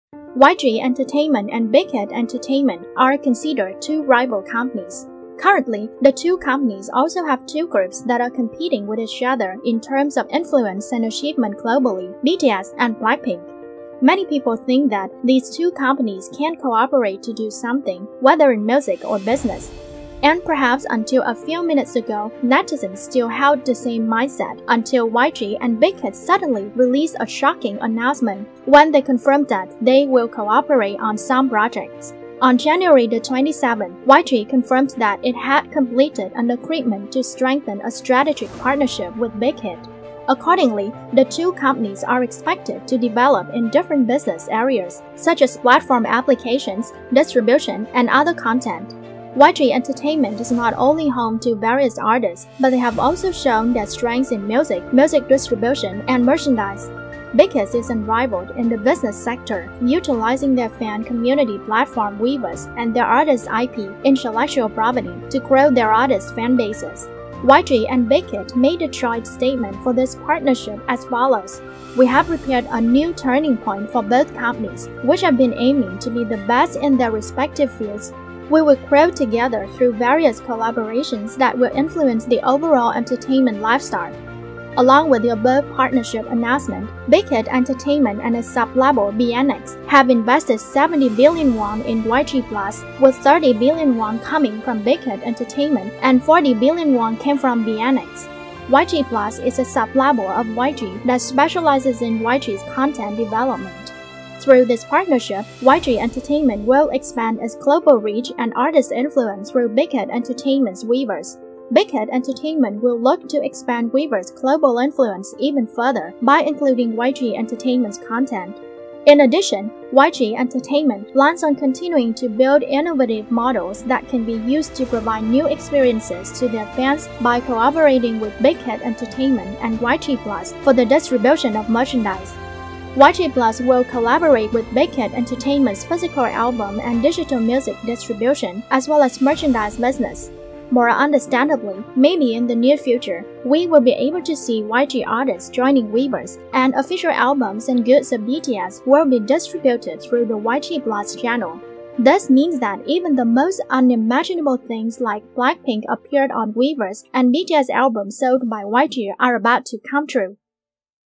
YG&BigHit confirmed cooperation: Will BP appear on Weverse, Will BTS's album be distributed by YG?
The news that YG Entertainment and Big Hit Entertainment form a joint business partnership makes Kpop fans surprised.